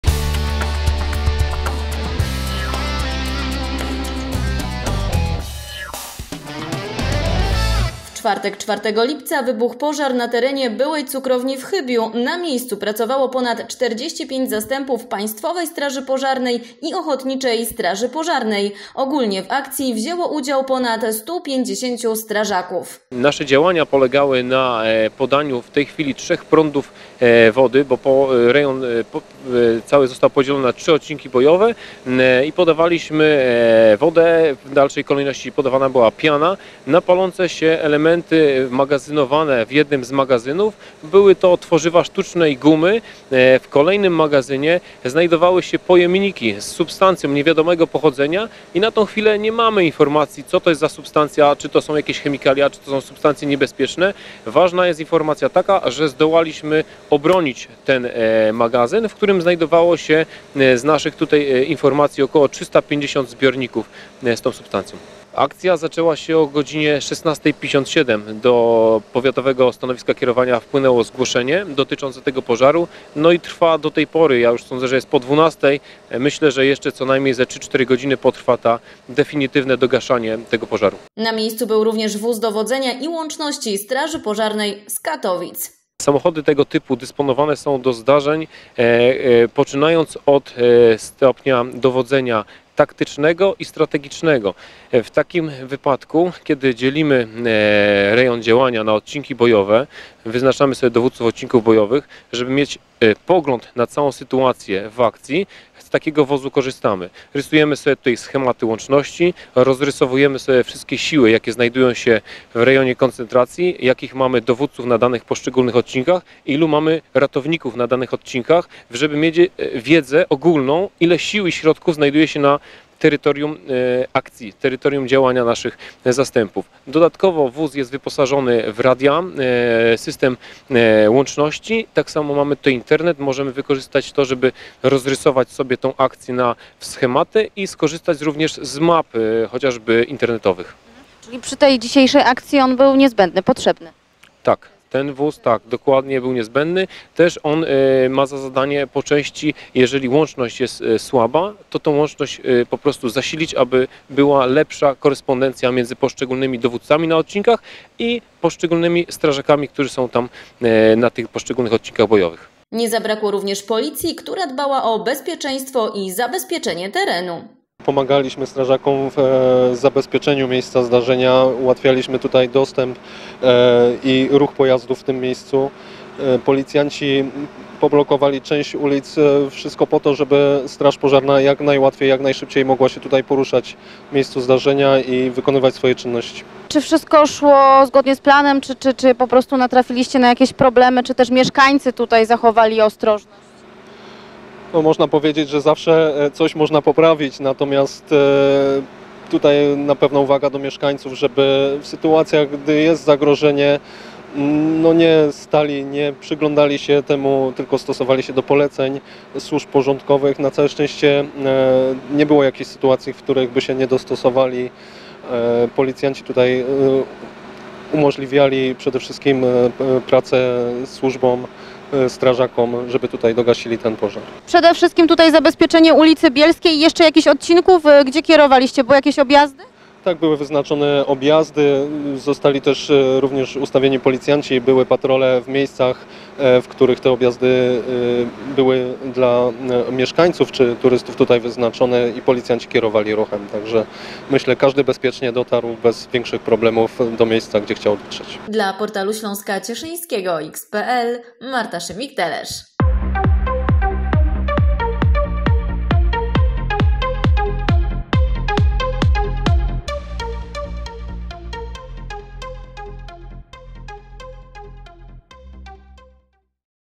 Pożar cukrowni w Chybiu
W czwartek (04.07) wybuchł pożar na terenie byłej cukrowni w Chybiu. Na miejscu pracowało ponad 45 zastępów Państwowej Straży Pożarnej i  Ochotniczej Straży Pożarnej. Ogólnie w akcji wzięło udział ponad 150 strażaków.